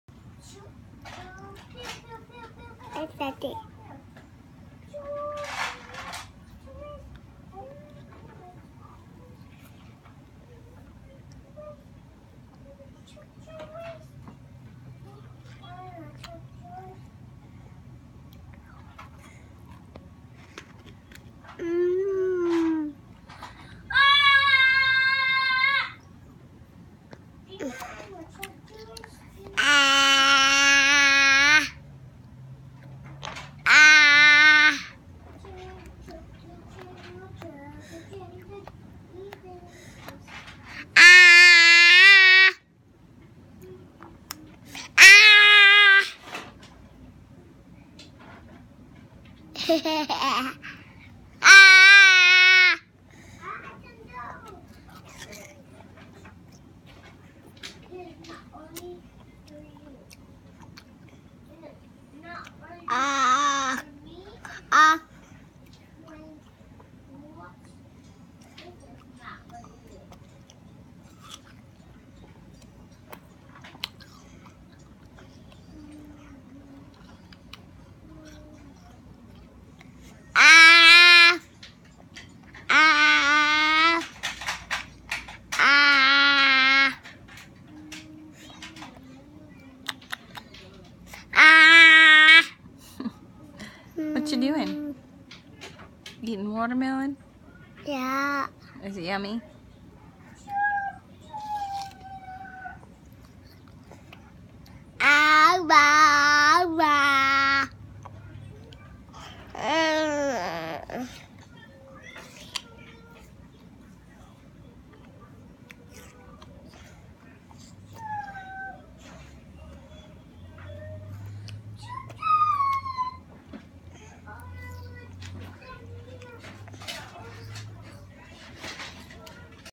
Watermelon
Jeremiah eating watermelon in a onesie made by his grandmother. Memorial Day weekend 2016